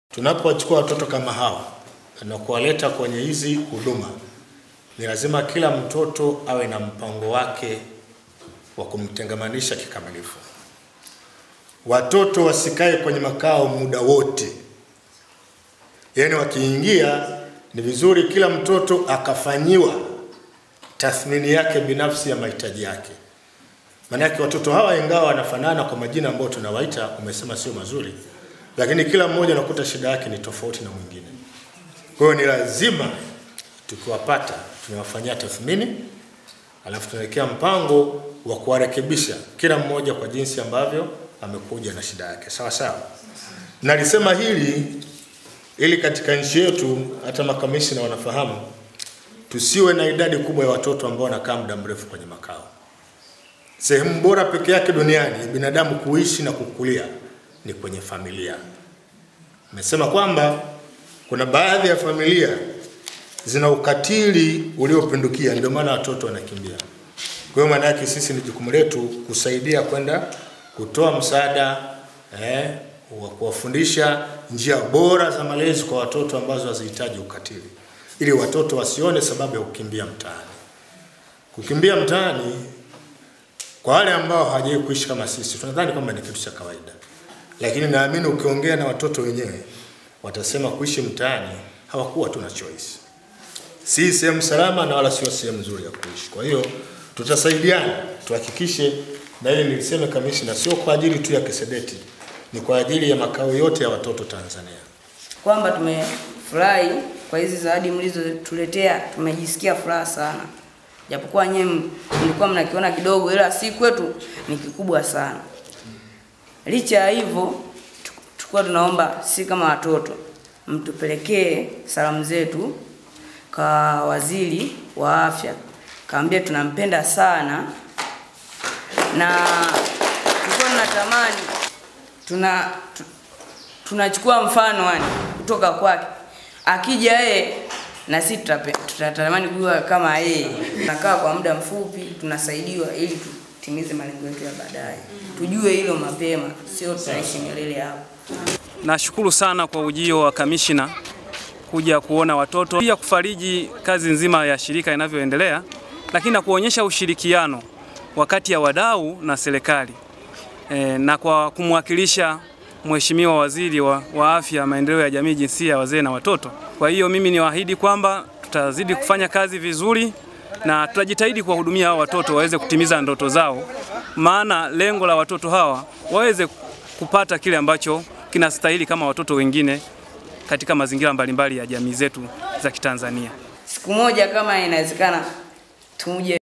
KAMISHNA NG'ONDI ATOA ANGALIZO KWA WAMILIKI WA VITUO VYA MALEZI YA WATOTO
Kamishna wa Ustawi wa Jamii Dkt. Naftali Ng'ondi akikabidhi zawadi za sikukuu ya Eid El Fitri kwa niaba ya Waziri wa Afya, Maendeleo ya Jamii, Jinsia, Wazee na watoto Dkt. Dorothy Gwajima kwa watoto wa kituo cha Kisedet jijini Dodoma, ametoa wito kwa wamiliki wa vituo kuhakikisha wanatimiza jukumu la kuwarejesha watoto kuungana na familia zao baada ya kutatua changamoto zilizowafanya wakimbilie mtaani.